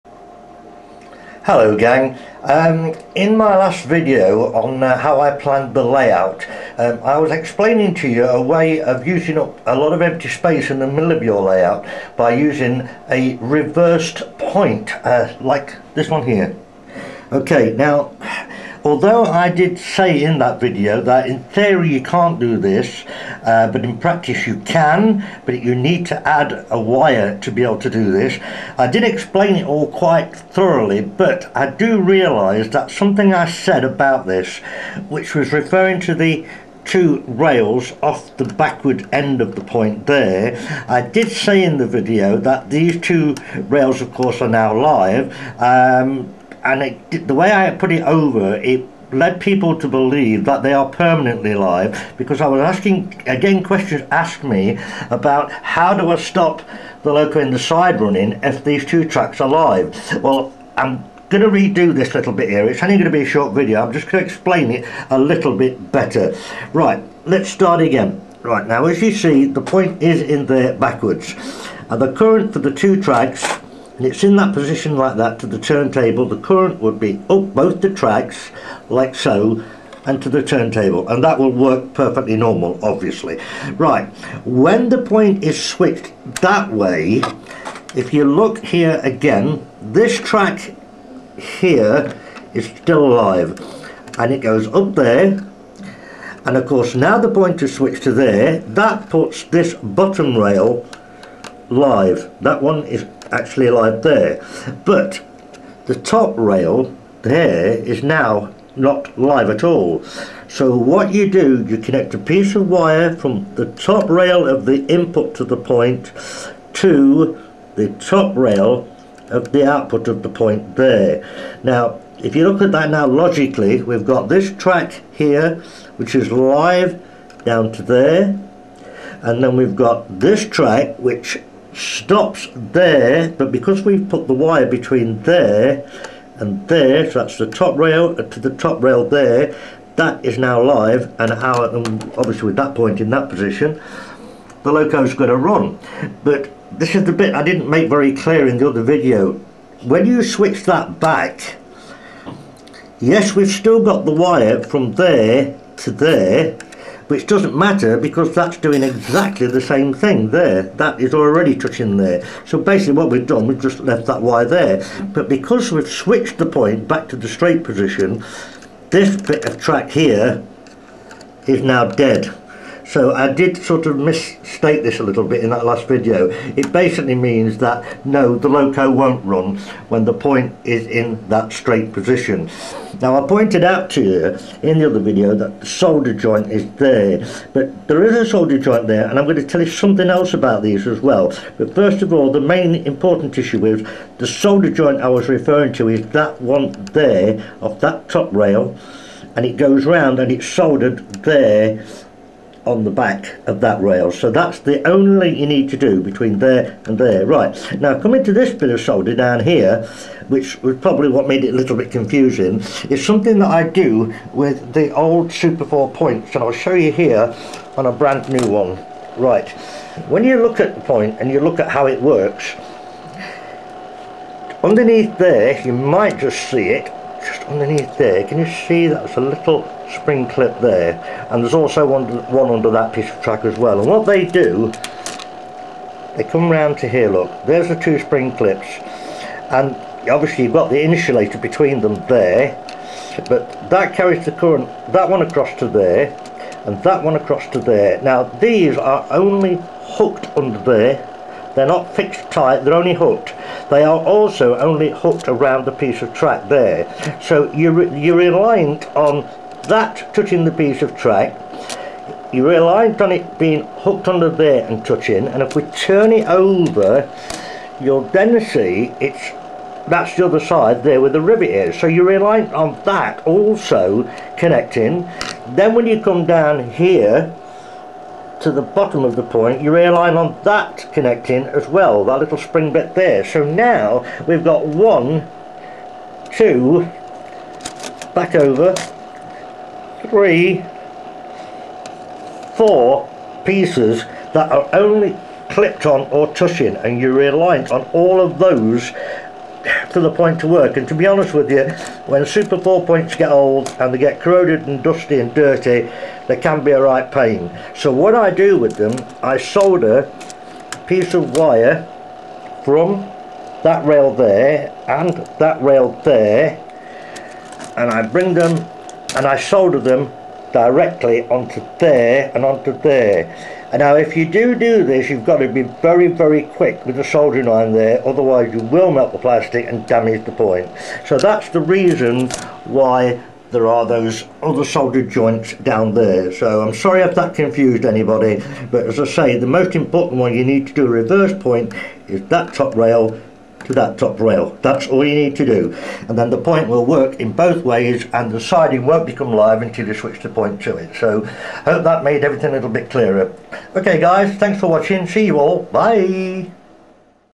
Reversed Point Wiring Update. Hornby Triang & All Brands Of Model Railway Track.
Hornby Triang Featuring: A more thorough explanation on the fitting & how to wire a reversed point. Remember this can be done with any brand pf points! Thanks for popping by. Cheers, John.